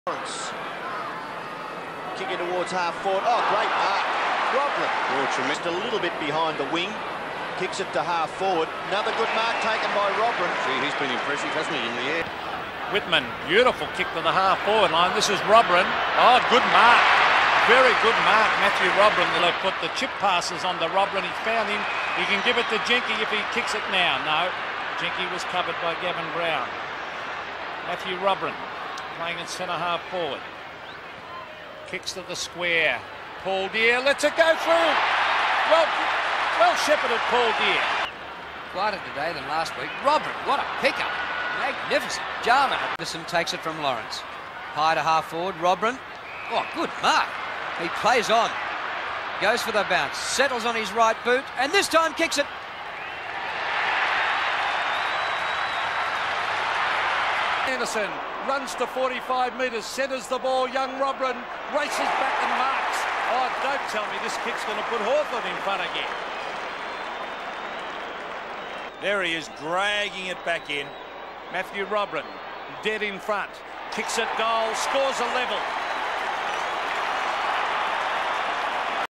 Matthew Robran - Hawthorn Highlights
Just the seven games in a premiership year, 1991.